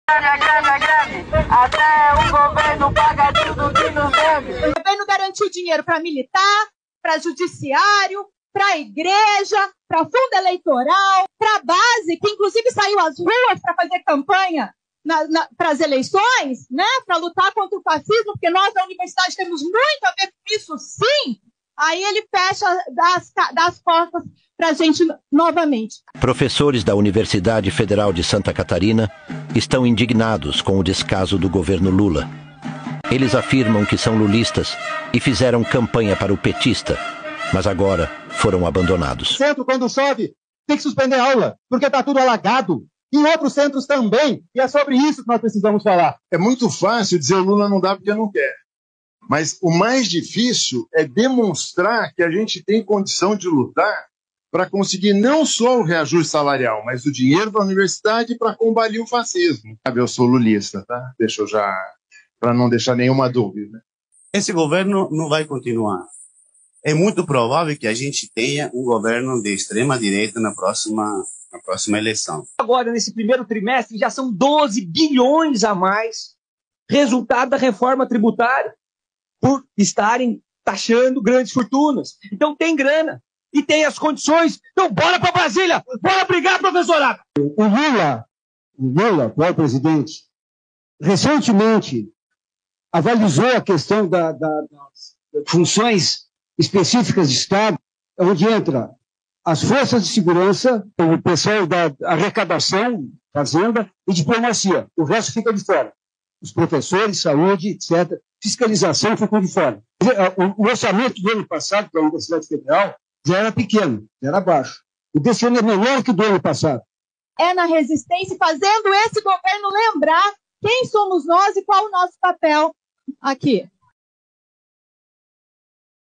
Professores da UFSC declaram greve...
Em Assembleia Geral Extraordinária (AGE) convocada pelo Sindicato dos Professores das Universidades Federais de Santa Catarina (Apufsc-Sindical), realizada na tarde de quarta-feira, 27 de março, os sindicalizados que fazem parte da categoria docente da Universidade Federal de Santa Catarina (UFSC) decidiram entrar em estado de greve. De acordo com a Apufsc, os professores reivindicam reajuste salarial em 2024: a categoria acumula cerca de 35% de defasagem nos salários, e a proposta do governo para este ano é de reajuste zero.

Na assembleia, que durou mais de três horas e foi promovida no auditório do Centro de Comunicação e Expressão (CCE), foram aprovados outros quatro encaminhamentos: criação de comissão de mobilização; nova AGE no dia 3 de abril, tendo a greve como pauta única; manifestação de apoio aos técnicos-administrativos em Educação (TAEs), que estão em greve desde 11 de março; e paralisação da categoria docente na próxima quarta-feira, 3 de abril, como parte de uma ação nacional que reivindica o reajuste salarial.

“O estado de greve não significa paralisação imediata dos trabalhos, portanto, os professores e professoras seguem trabalhando normalmente. O estado de greve é um alerta aprovado por uma categoria de trabalhadores, anunciando que uma greve poderá ser deflagrada”, comunica a entidade em nota. “A diretoria da Apufsc ressalta que a greve deve ser o último recurso, quando todas as negociações já se esgotaram, e elas ainda estão acontecendo, com a expectativa de uma nova mesa de negociação na primeira quinzena de abril”, traz o texto.

Com informações da Comunicação da Apufsc.